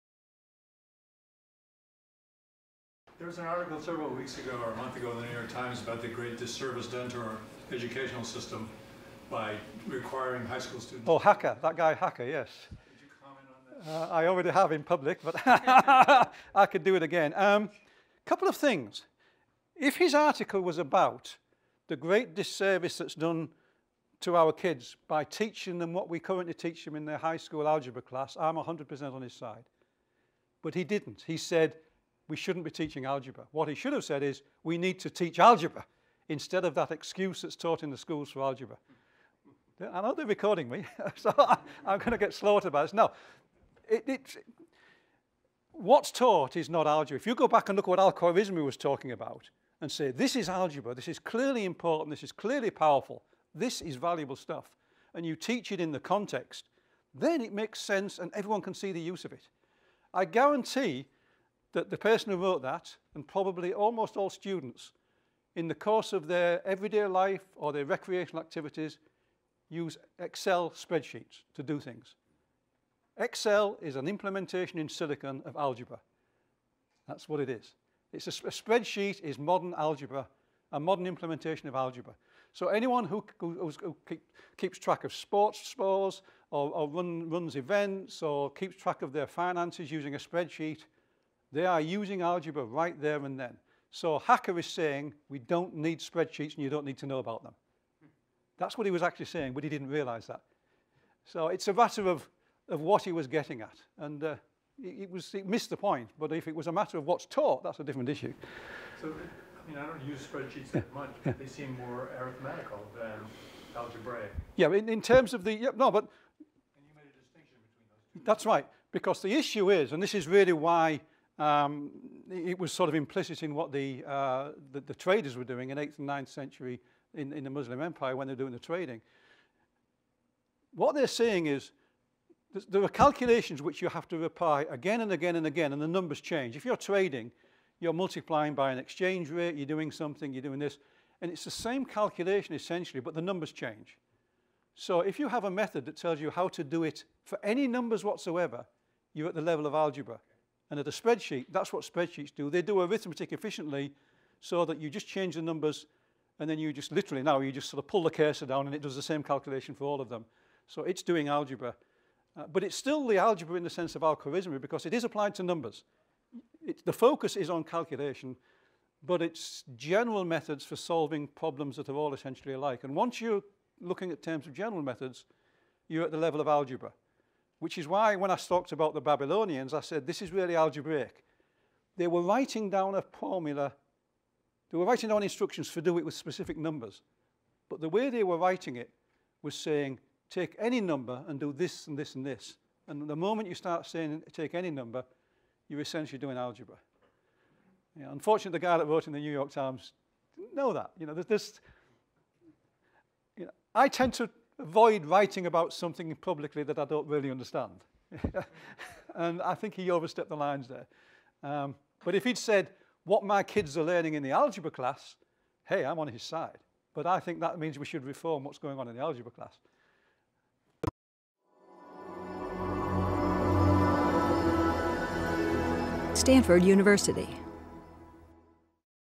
Q&A: Why we teach algebra
In 2012, Andrew Hacker wrote an opinion piece in the New York Times Magazine suggesting that teaching algebra in high school was a waste of time. I was asked to comment on Hacker's article by students in my Stanford Continuing Studies course "Mathematics: Making the Invisible Visible" on iTunes University I did not mince my words. Hacker was displaying his ignorance of what algebra is.